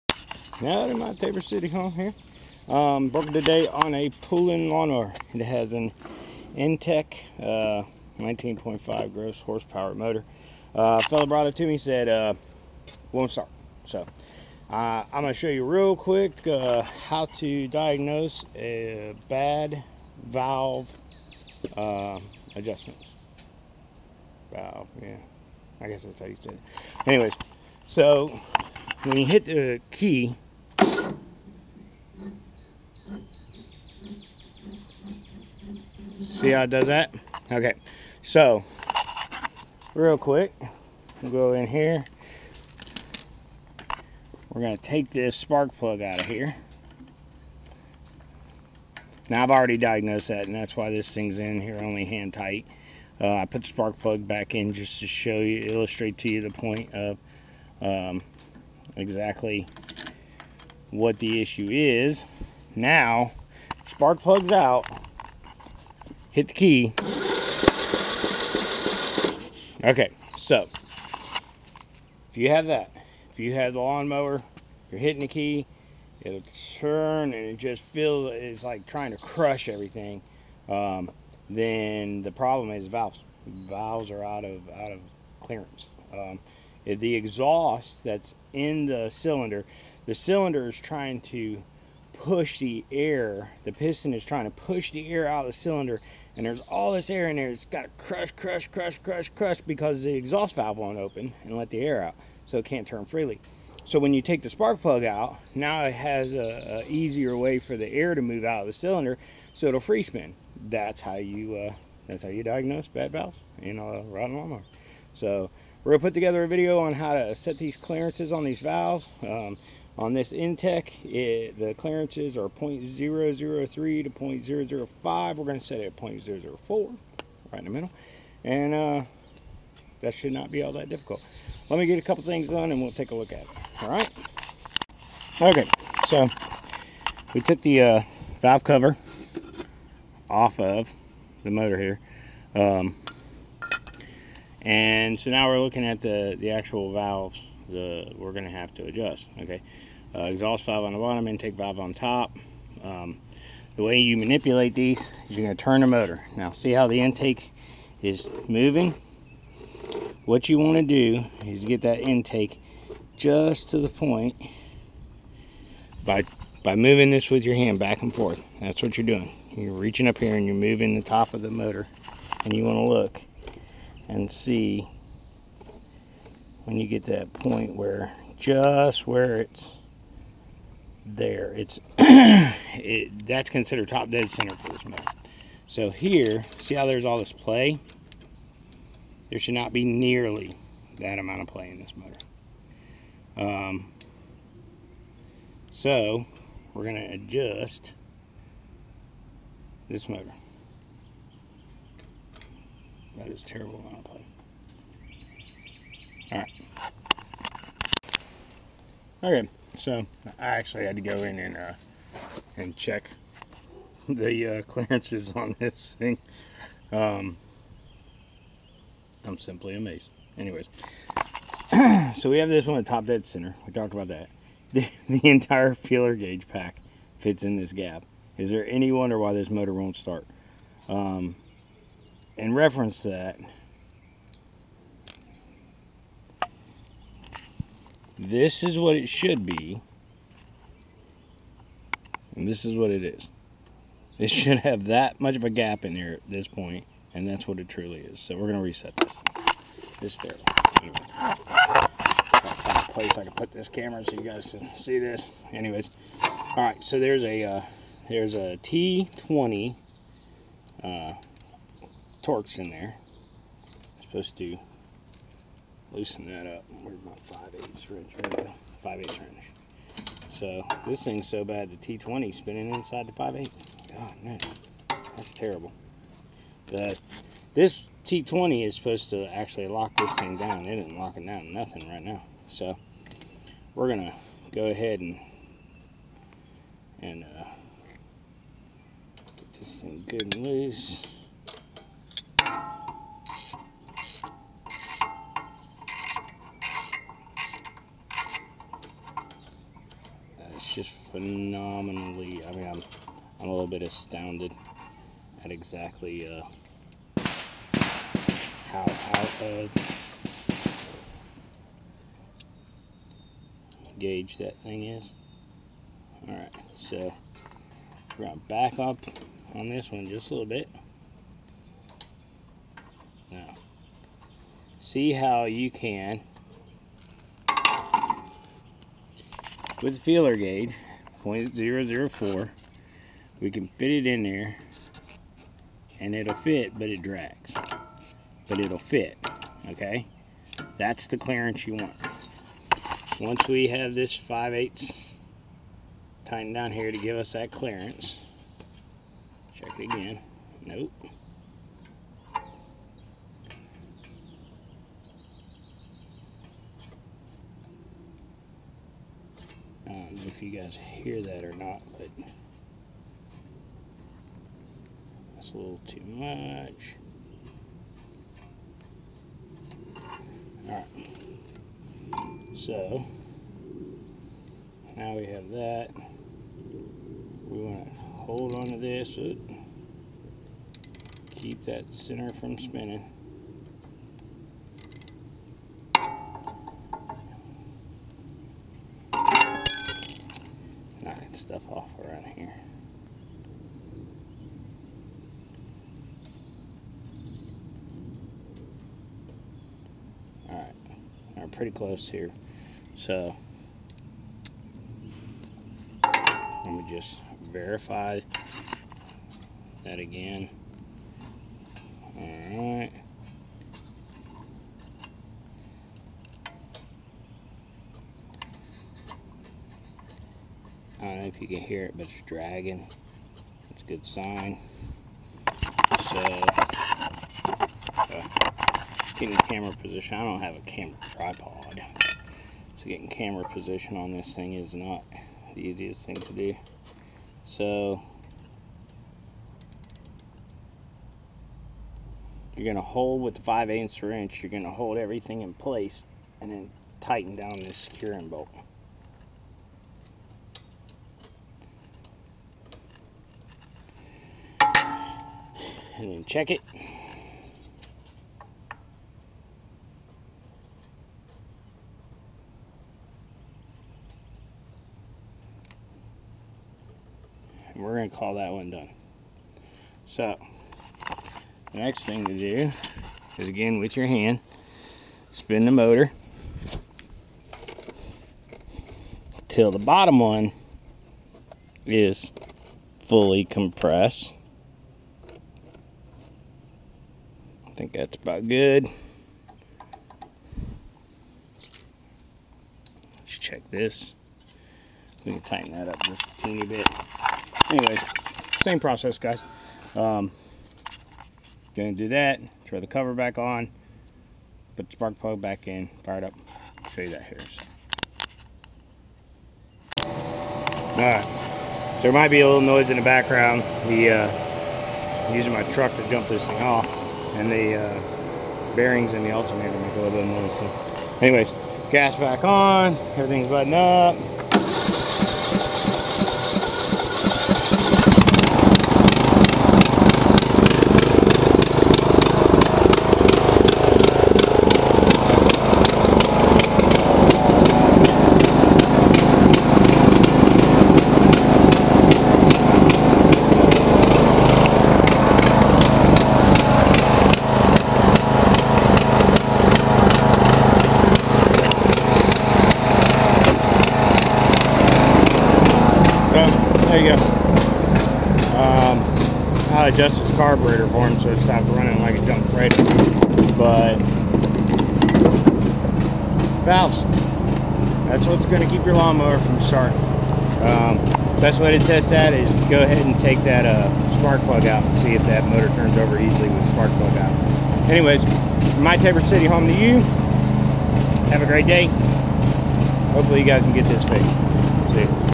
Briggs and Stratton INTEK 19.5HP motor not starting (How to fix a Valve Clearance Issue)
This Briggs and Stratton INTEK 19.5hp motor was brought to me by a fella with a slow turning motor that wouldn't turn over.  He had replaced the battery and thought he had a starter problem.  It was actually the valve clearances which were the issue.  The video shows how to diagnose and resolve a valve clearance issue with this style motor.